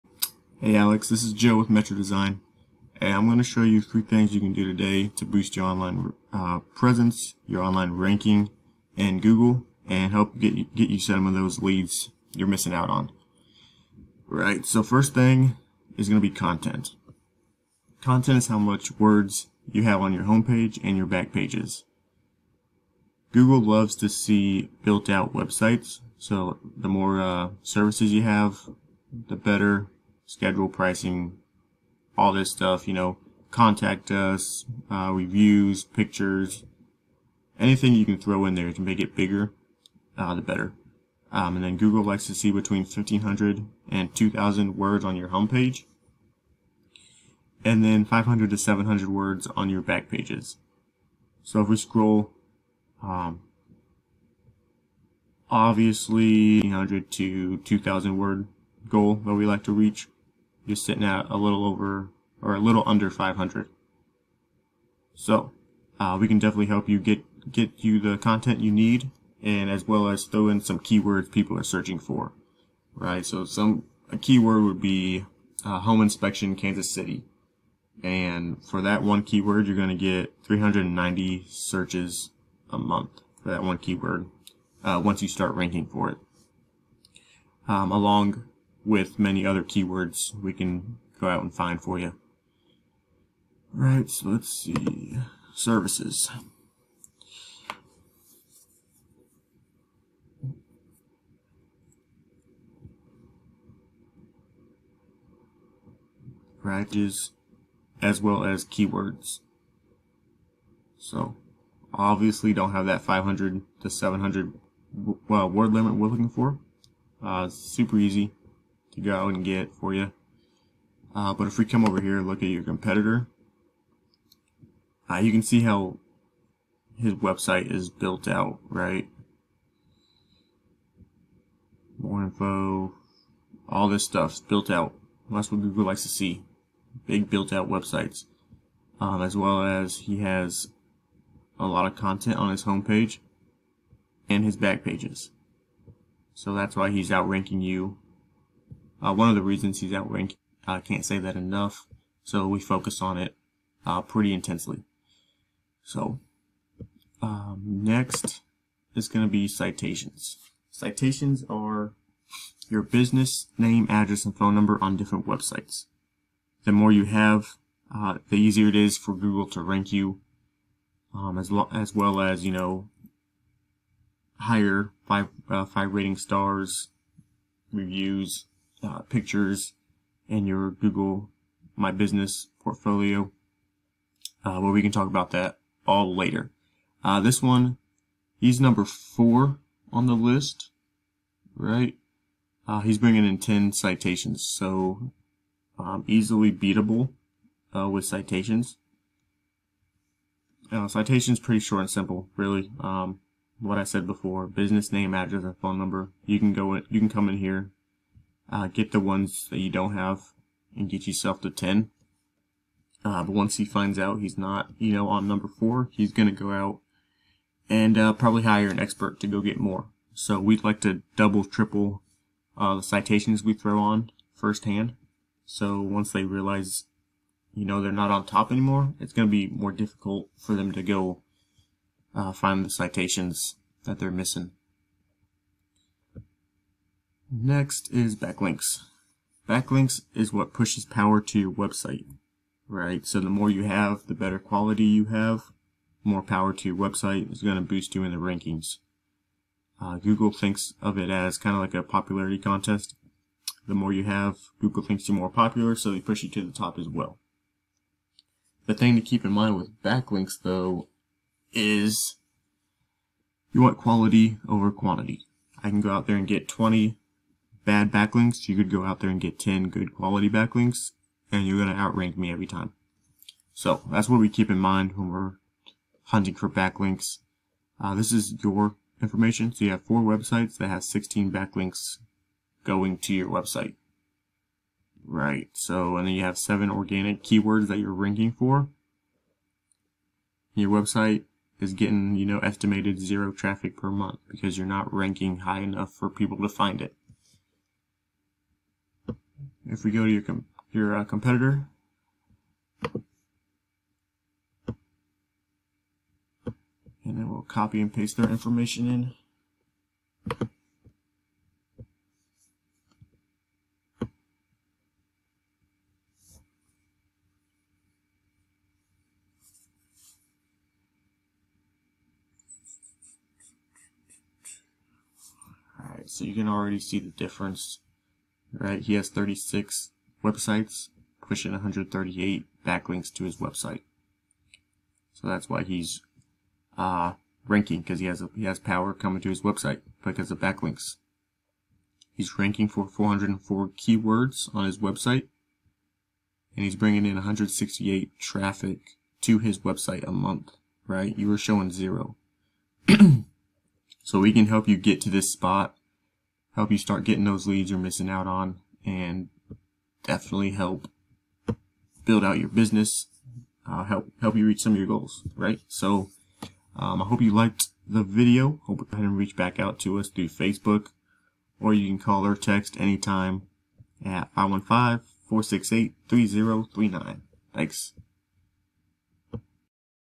aclarkinspections
SEO help from Metro Design showing what is needed to rank on Google for "house inspections kc"